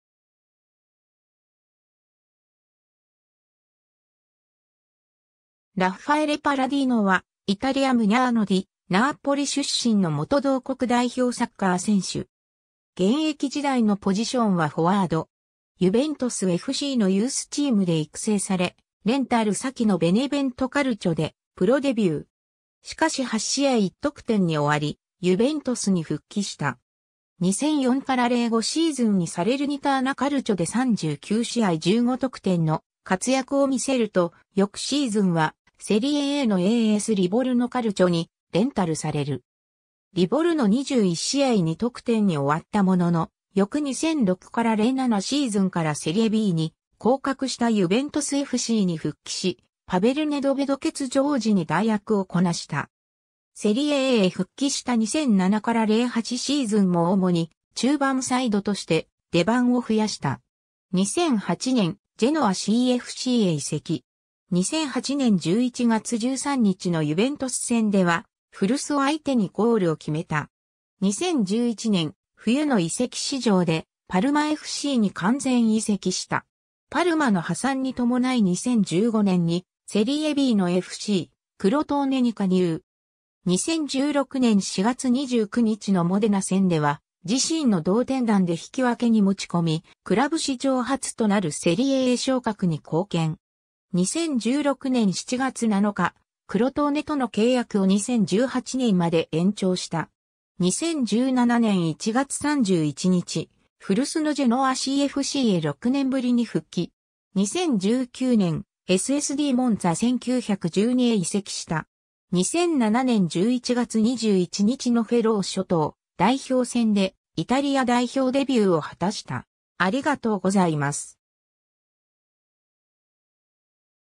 ラッファエレ・パッラディーノ
ベネヴェント・カルチョの選手
ASリヴォルノ・カルチョの選手
スペツィア・カルチョの選手
ラッファエレ・パッラディーノ（Raffaele Palladino, 1984年4月17日 - ）は、イタリア・ムニャーノ・ディ・ナーポリ出身の元同国代表サッカー選手。
 ユヴェントスFCのユースチームで育成され、レンタル先のベネヴェント・カルチョでプロデビュー。
しかし8試合1得点に終わり、ユヴェントスに復帰した。
2004-05シーズンにサレルニターナ・カルチョで39試合15得点の活躍を見せると、翌シーズンはセリエAのASリヴォルノ・カルチョにレンタルされる。
リヴォルノ21試合2得点に終わったものの、翌2006-07シーズンからセリエBに降格したユヴェントスFCに復帰し、パベル・ネドベド欠場時に代役をこなした。
セリエAへ復帰した2007-08シーズンも主に中盤サイドとして出番を増やした。
 2008年、ジェノアCFCへ移籍。
2008年11月13日のユヴェントス戦では古巣を相手にゴールを決めた。
 2011年、冬の移籍市場でパルマFCに完全移籍した。
 パルマの破産に伴い2015年にセリエBのFCクロトーネに加入。
2016年4月29日のモデナ戦では自身の同点弾で引き分けに持ち込み、クラブ史上初となるセリエA昇格に貢献。
2016年7月7日、クロトーネとの契約を2018年まで延長した。
 2017年1月31日、古巣のジェノアCFCへ6年ぶりに復帰。
 2019年、SSDモンツァ1912へ移籍した。
 2007年11月21日のフェロー諸島代表戦でイタリア代表デビューを果たした。